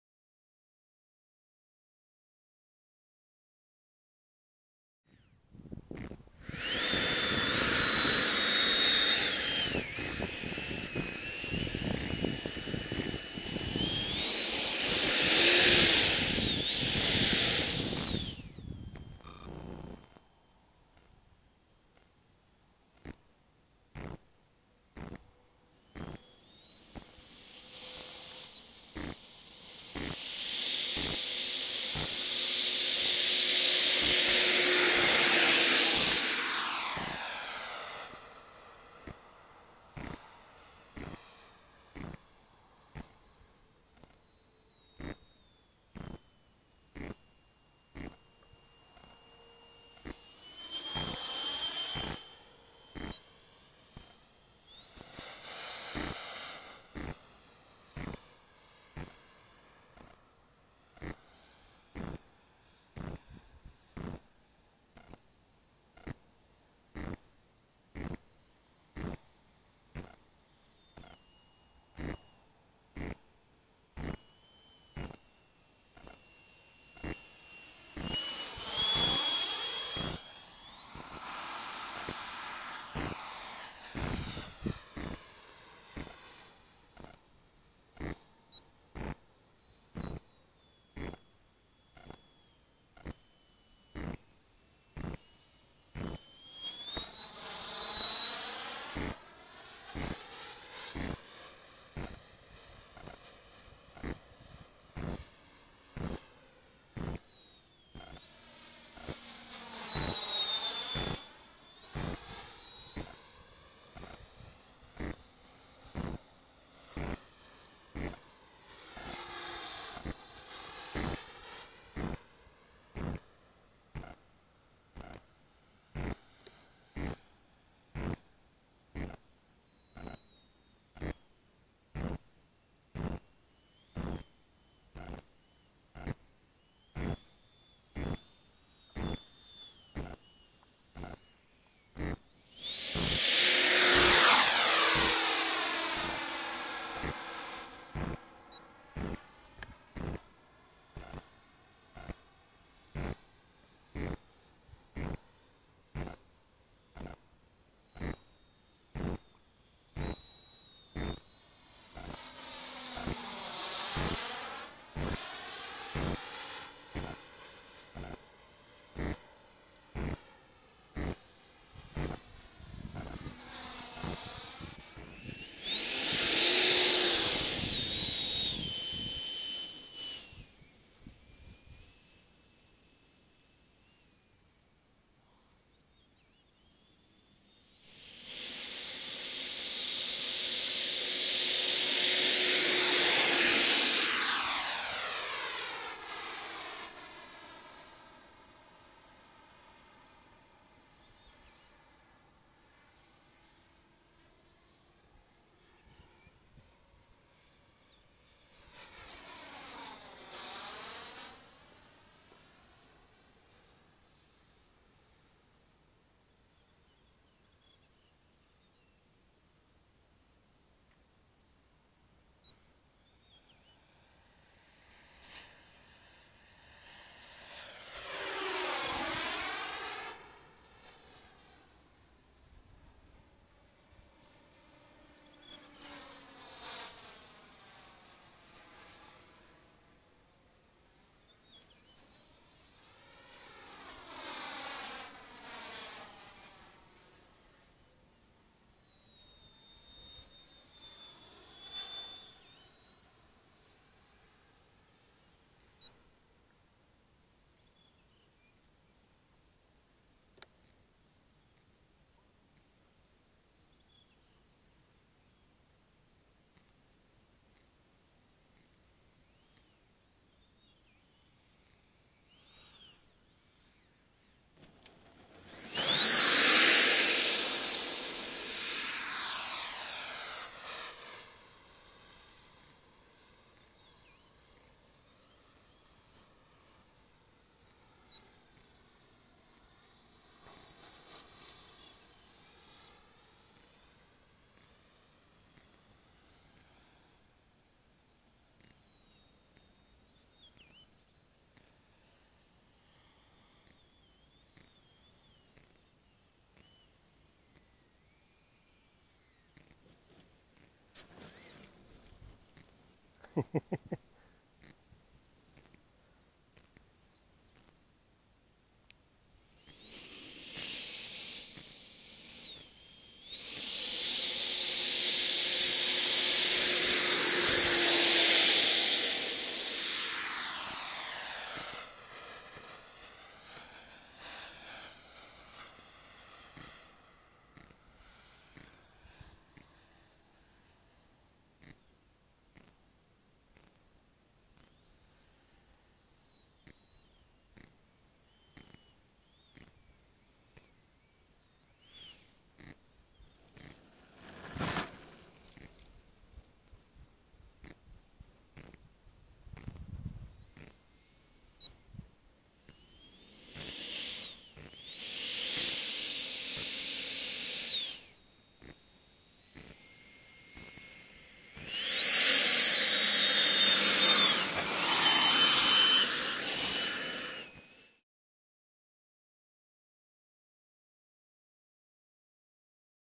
Big RC Jet Scary Slow Flying - EL 39 3D Printed Jet
Flying my big EL-39 jet slow enough to scare people.

Sorry about the weird droning sound. My camera is from 2016 and has served me well over the years but now it's dying.